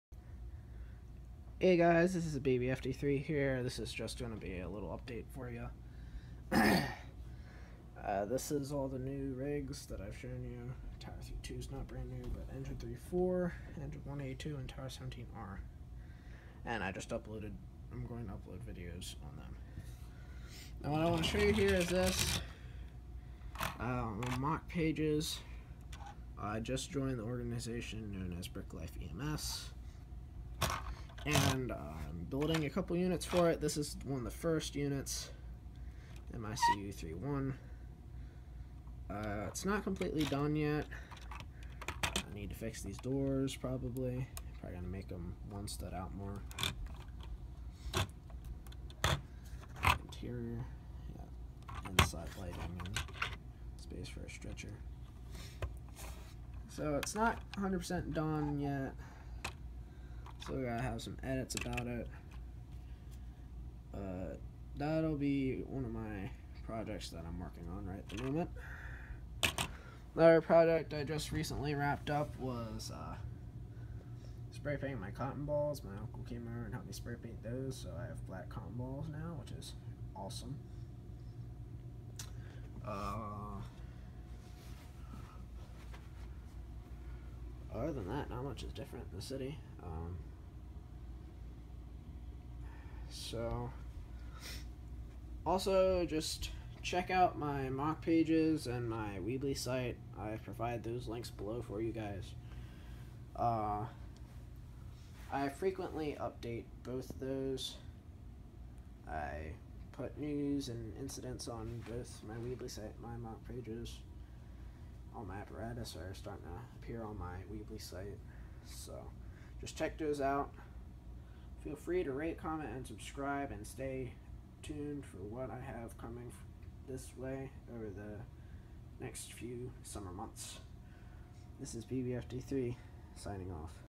Update #3 BrickLife EMS
In this update, I show you a little of what I've been up to as of late, and I give you a sneak peek of one of my works in progress. To learn more, go to the links provided below: